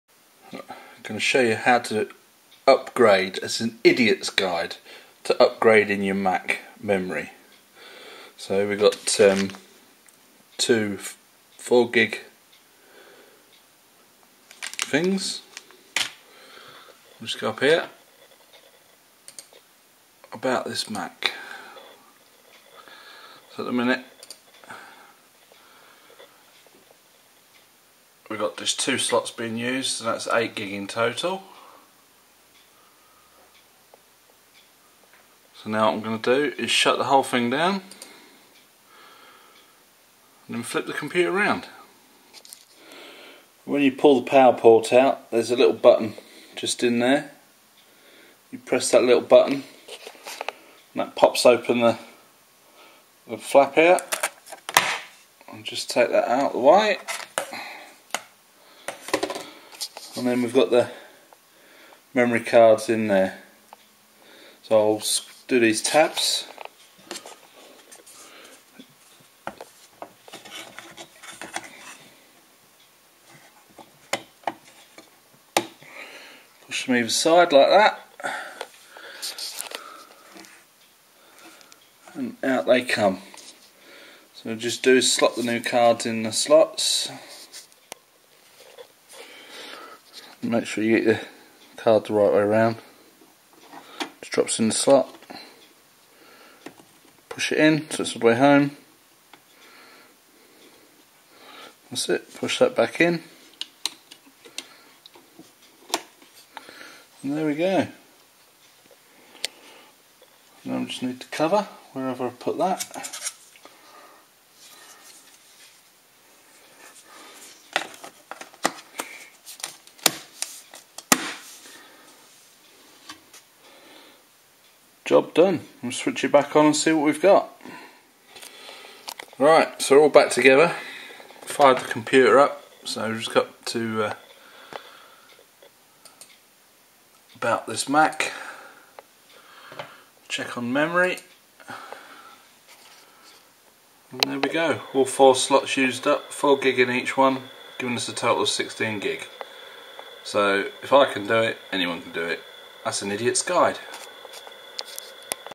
Upgrading memory on iMac (Retina 5k, 27inch, mid 2015)
Upgrading memory on iMac (Retina 5k, 27inch, mid 2015) // Adding extra memory to iMac. Normally running the iMac with quite a few large programs at the same time. It came from the shop with  just 8gb of memory and just two slots being used. Decided to upgrade so added another 8gb.Here is an idiots guide to doing the job.

If your looking to upgrade your ram, please look